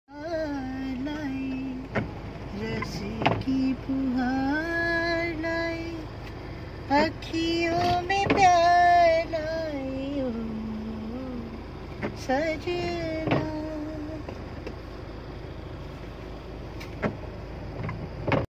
Embark on a thrilling rain-soaked adventure! Explore the majestic Western Ghat!!!(3)
Embark on a thrilling rain-soaked adventure! Explore the majestic Western Ghat in pursuit of a breathtaking waterfall.